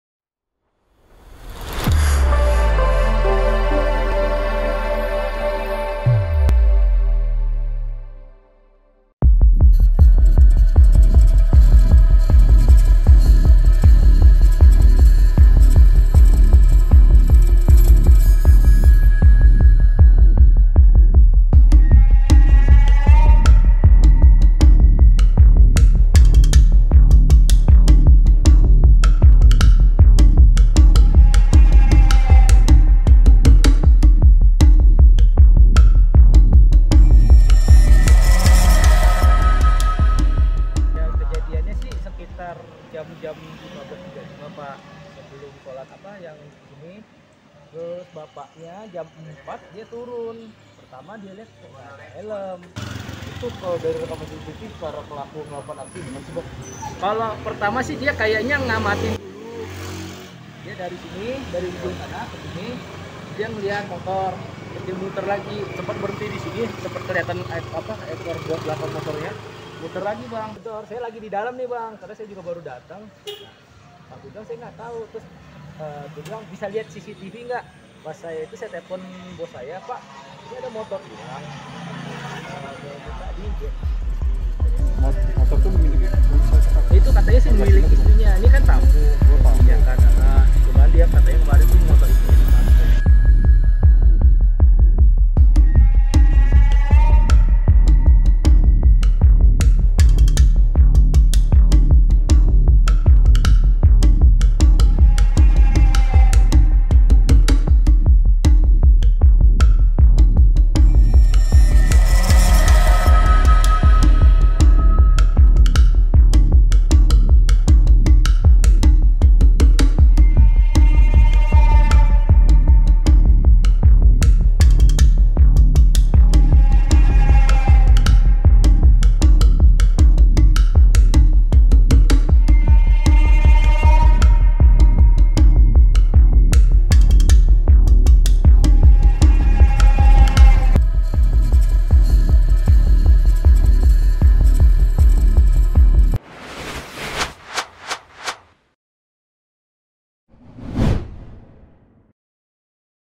AKSI DUA MALING MENGGASAK SEPEDA MOTOR DI KOJA TEREKAM CCTV
JAKARTA,POSKOTA.CO.ID - Aksi pencurian kendaraan bermotor di Jalan Cipeucang 2 Nomor 3, Lagoa, Koja, Jakarta Utara terekam kamera CCTV, pada Selasa (20/4/2021) sore.

Dalam rekaman CCTV, sebelum melancarkan aksinya kedua maling yang berboncengan menggunakan motor matik warna pink terlihat mondar mandir di lokasi seperti sedang mengamati situasi.

Meski panas terik, tampak situasi di lokasi jalan tersebut sepi dari aktifitas masyarakat.

Setelah dinilai aman, satu maling yang mengenakan helm dan jaket hitam berlengan putih mendekati 4 motor yang terparkir di depan rumah kantor (rukan).

Sedang satu pelaku yang bertugas sebagai joki, menunggu di atas motornya sambil mengamati situasi.

Sejenak, si maling yang bertugas menjadi eksekutor terlihat duduk di atas motor incarannya, sambil mungutak atik rumah kunci kendaraan roda dua tersebut.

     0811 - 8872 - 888